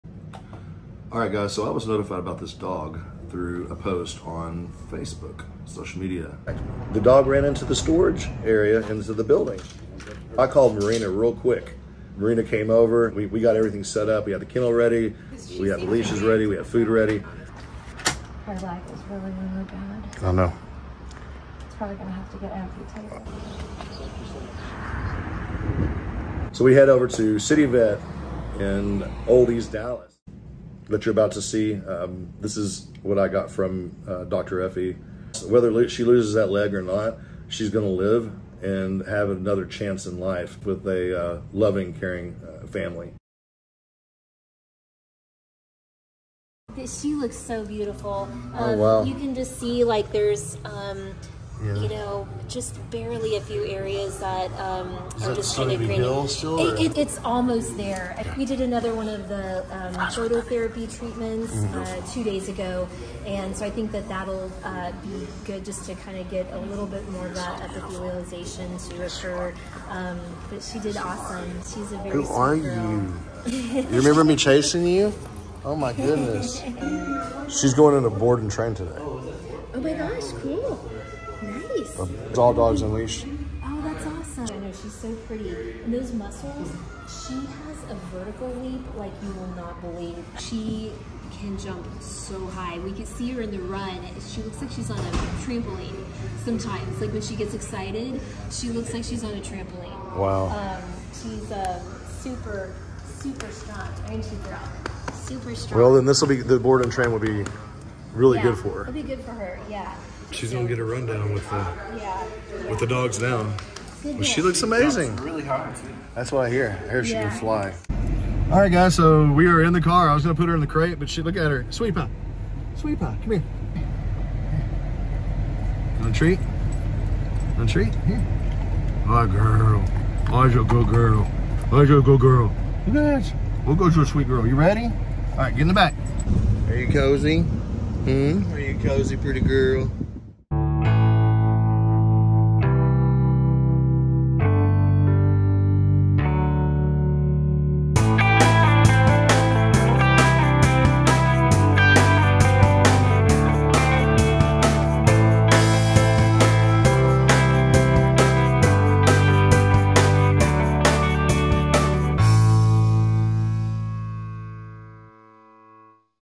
Saving Sweetie Pie
Another dog left on the streets to fend for herself.